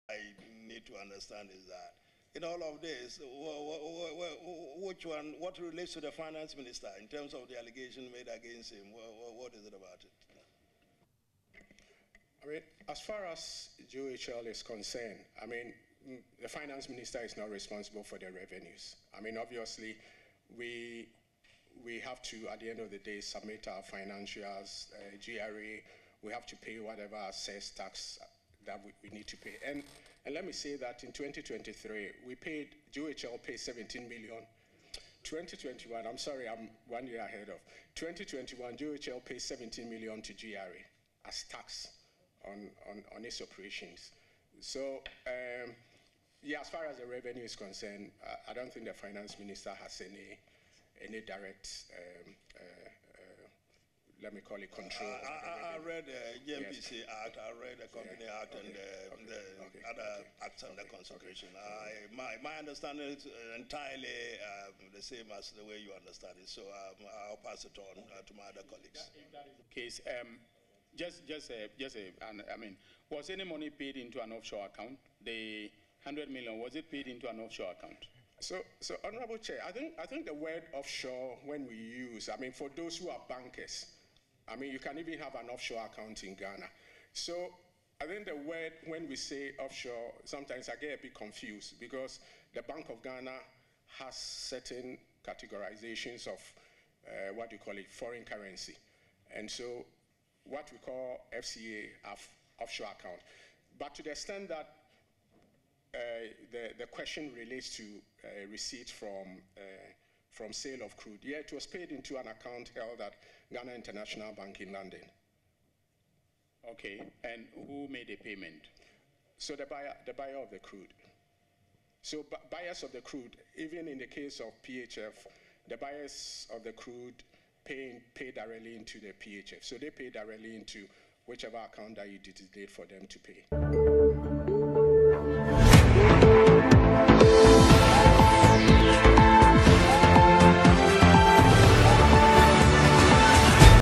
Censure motion hearings: Finance Minister not involved in financials of this matter. - GNPC
Censure motion hearings: Finance Minister Ken Ofori-Atta not involved in financials of this matter. - GNPC